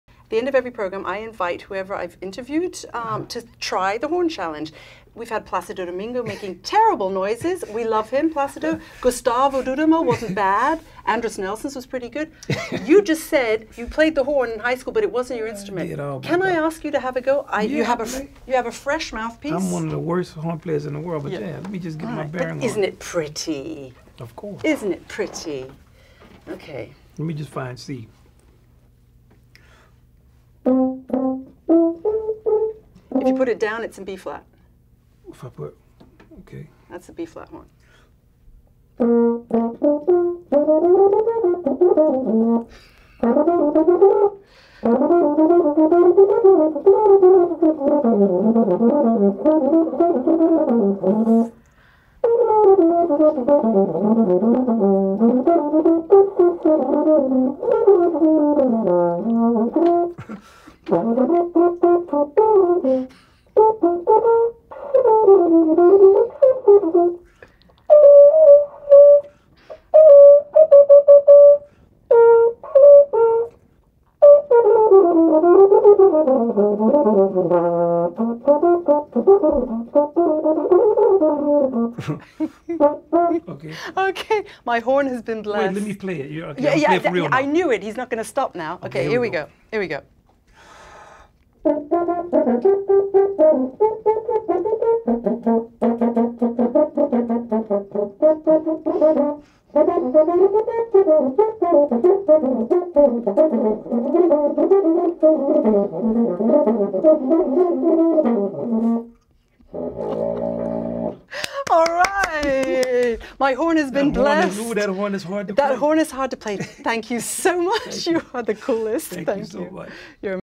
Wynton Marsalis takes the Sarah´s Music Horn Challenge
Sarah´s horn has been blessed by a master! Watch the whole Sarah´s Music episode featuring Wynton and his Jazz at Lincoln Center Orchestra